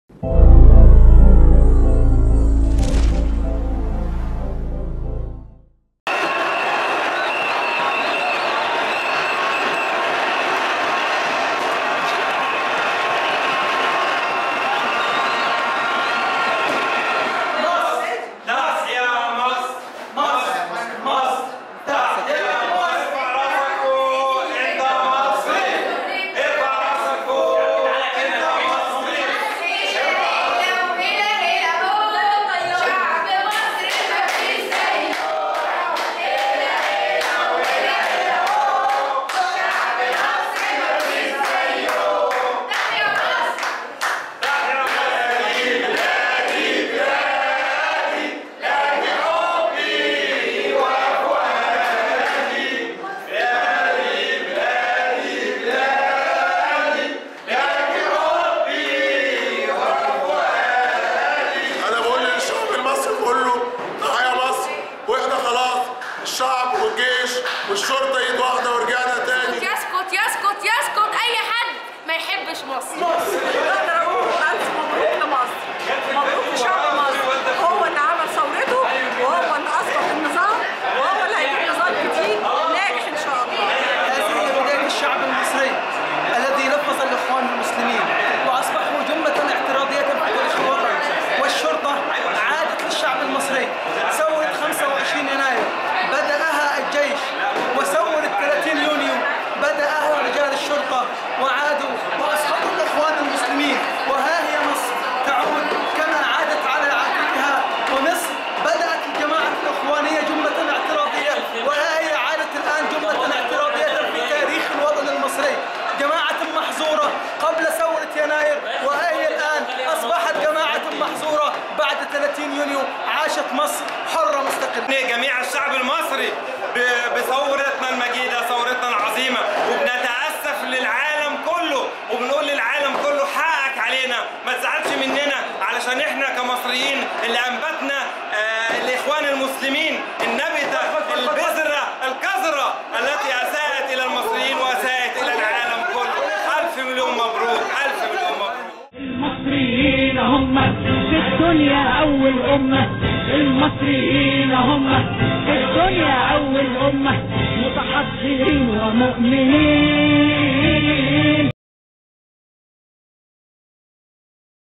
صحفيو "أخبار اليوم" يحتفلون برحيل نظام الإخوان
صحفيو مؤسسة أخبار اليوم يحتفلون إبتهاجا ببيان القيادة العامة للقوات المسلحة المصرية ورحيل نظام الإخوان ويوجهون كل التحية والتقدير لرجال القوات المسلحة البواسل على دورهم العظيم فى الحفاظ على مصر والعبور بها من أخطر الأزمات التى مرت بمصر ... حفظ الله مصر ... حفظ الله جيش مصر العظيم .

ياسر حماية - محمد فايد - محمد فتحى - إبراهيم حسين - إيهاب على - دعاء فوده - نبيلة صبيح - مى عبد العزيز .